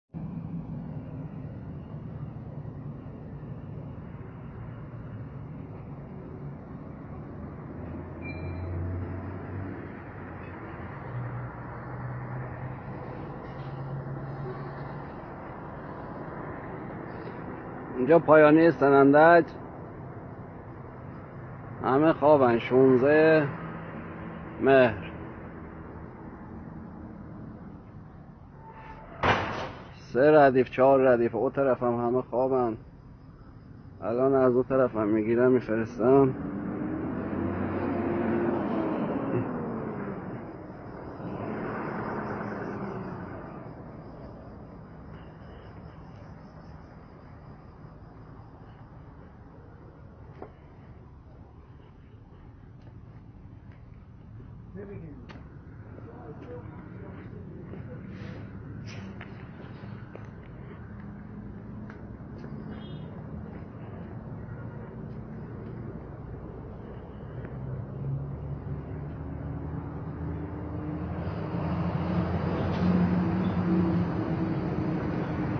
99
دوشنبه ۱۶ مهر ۹۷ 🌐 سنندج.
اعتصاب کامیونداران